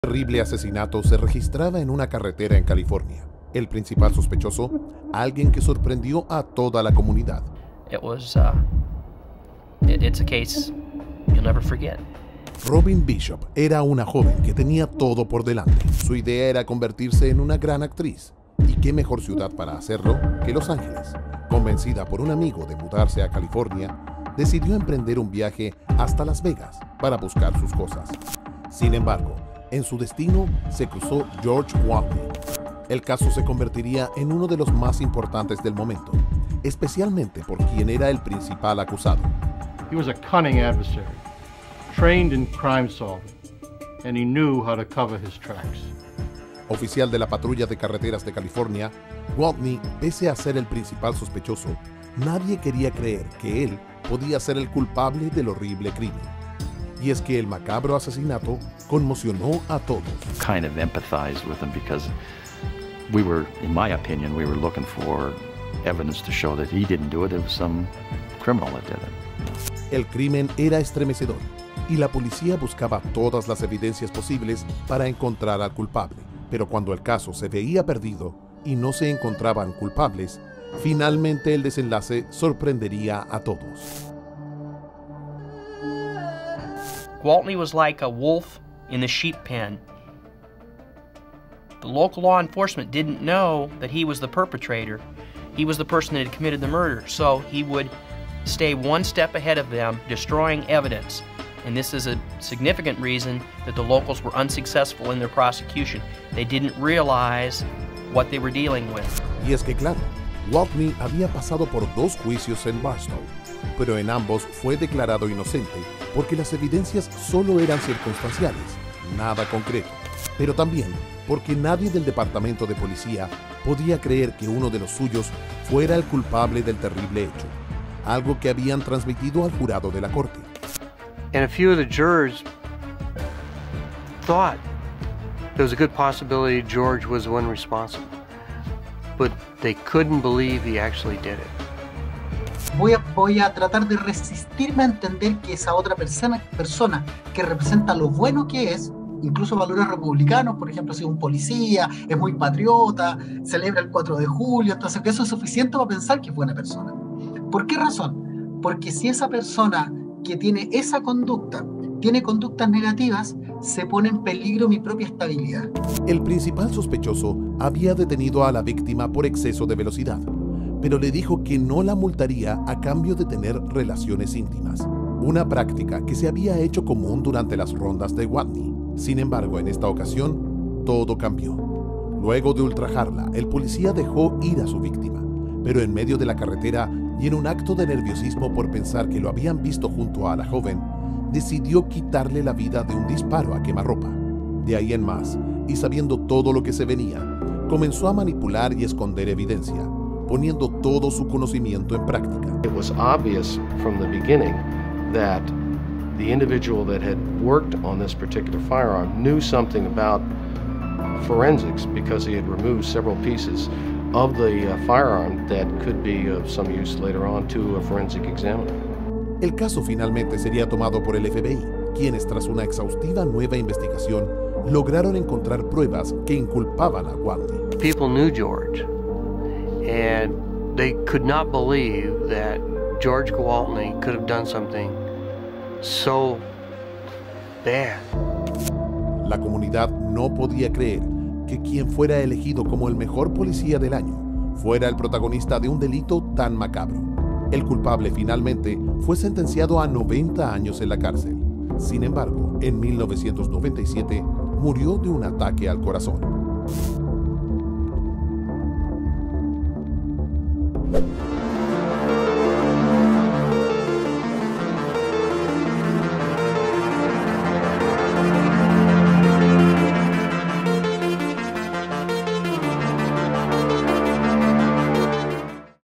El policía que creyó estar por sobre la ley
Viajó a Los Angeles buscando convertirse en actriz pero se topó con un monstruo en la autopista...
La policía no pudo creerlo cuando se dieron cuenta quién era ese hombre.